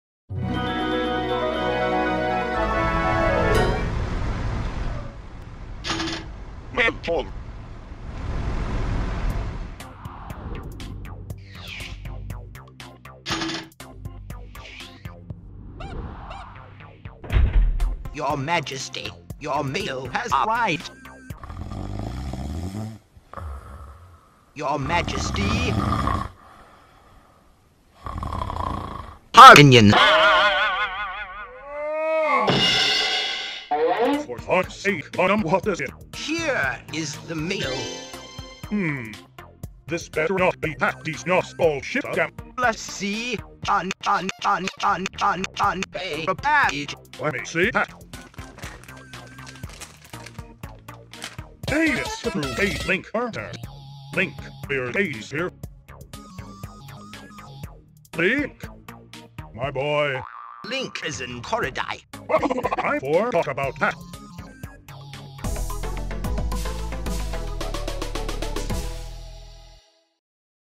YTPWare Shit Together! Opening Cutscene Preview
When a new game arrives at the castle, the curious King Harkinian decides to play it first before Link returns from Koridai. The stakes get higher when Gwonam has a go at the game and the king bets rubies on his score.

"YTPWare: Sh*t Together!" is going to be my first fully animated YTP in a similar vein to WarioWare Poop Moves, rather than The Many Microgame$ of Winnie The Pooh, which used mostly unedited footage. Some sprites were made from scratch by me, while others are edited versions of existing sprites.

Feedback would be very much appreciated as this is my first time doing an animated/flash poop.